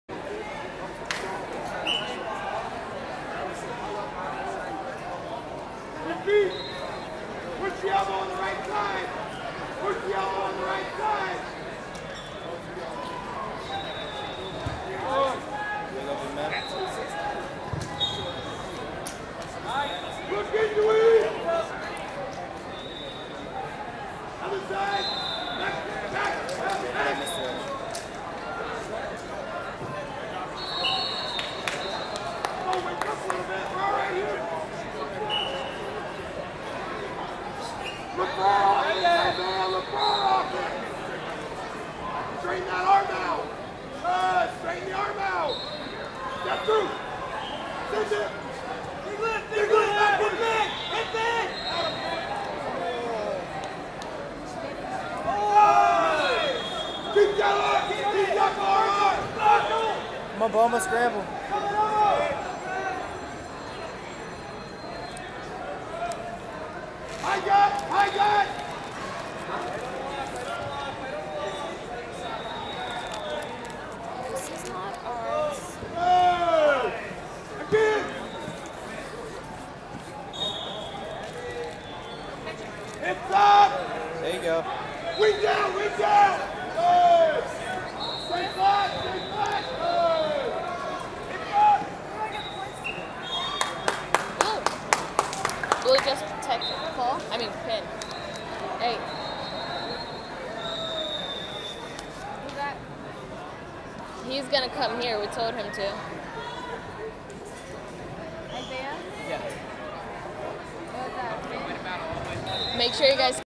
120 Kg Quarterfinal - Brayden Bouma, Tennessee Vs Isaiah Graves, UTB Wrestling 35ee
FloSports/2021 Southeast Regionals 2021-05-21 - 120 Kg Quarterfinal - Brayden Bouma, Tennessee Vs Isaiah Graves, UTB Wrestling 35ee